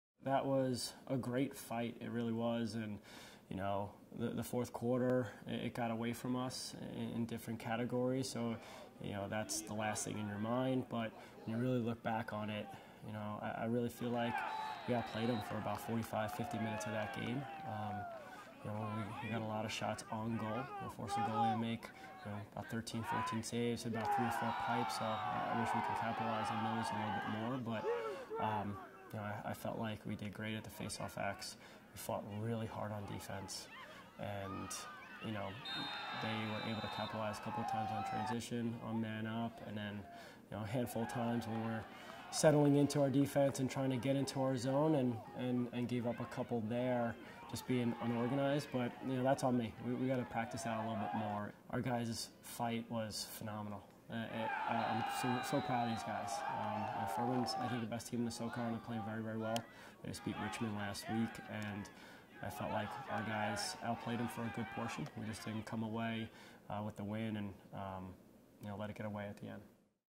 Furman vs VMI Recap
VMI falls to Furman 11 to  7 after a tough battle.